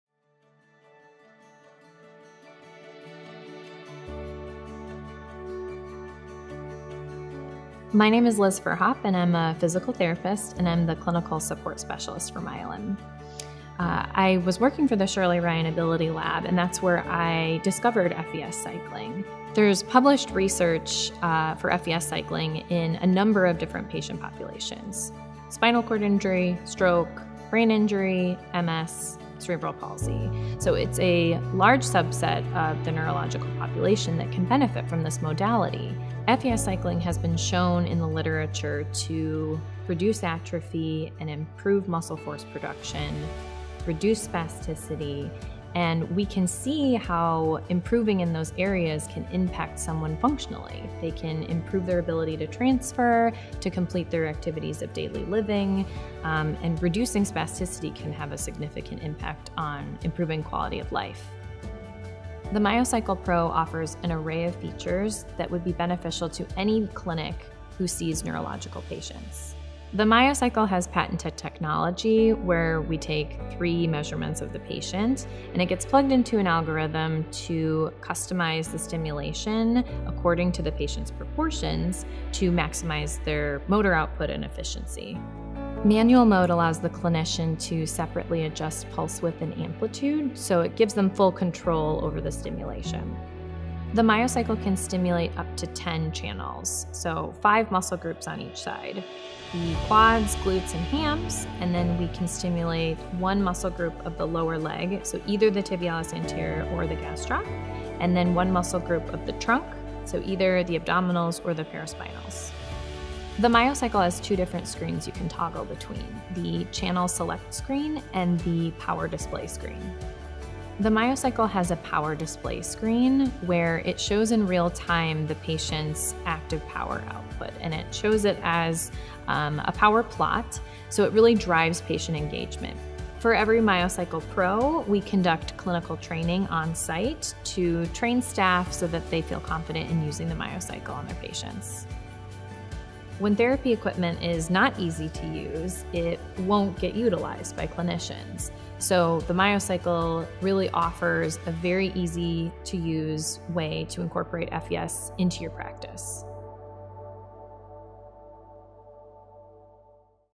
The MyoCycle Pro: Perfect For Clinics
The MyoCycle Pro is the perfect tool for any clinic that works with neuro patients. With features specifically designed for the clinical setting, the MyoCycle Pro provides state-of-the-art FES therapy in an easy-to-use way.

FES cycling therapy with the MyoCycle is FDA-cleared and proven to:
-provide neuromuscular re-education
-prevent or reverse atrophy
-increase range of motion
-increase local blood flow
-reduce muscle spasms

Visit MYOLYN.com to learn more!